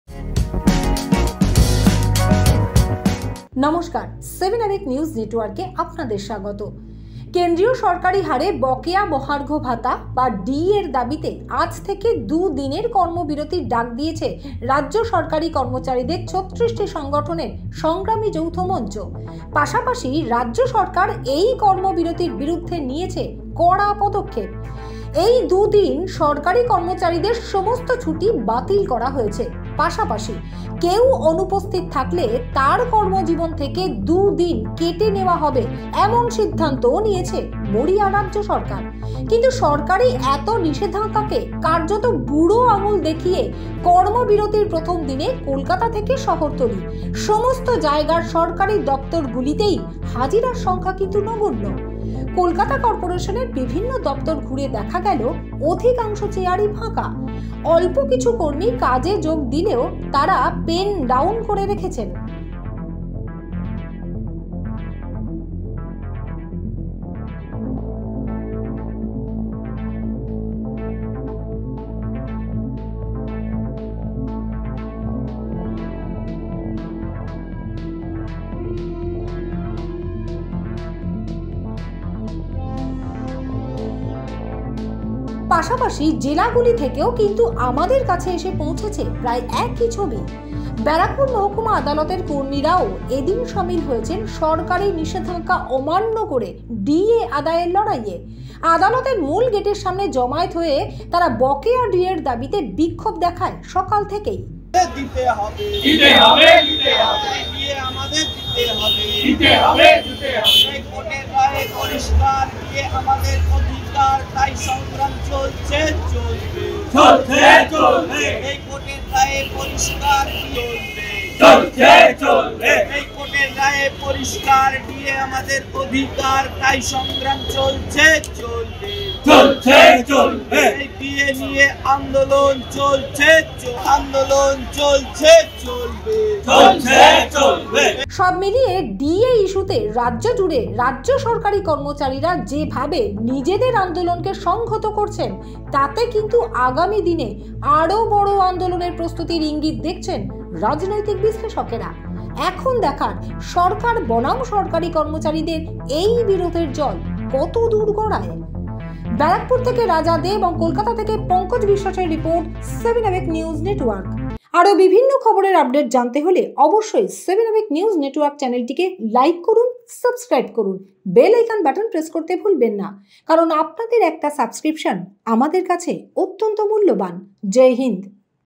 DA : চাকরি জীবনে ছেদ, নির্দেশিকা অমান্য করেই রাজ্য জুড়ে চলছে সরকারী কর্মীদের ডিএ'র দাবীতে কর্মবিরতি
চাকরি জীবনে ছেদ, নির্দেশিকা অমান্য করেই রাজ্য জুড়ে চলছে সরকারী কর্মীদের ডিএ'র দাবীতে কর্মবিরতি

Jai HInd!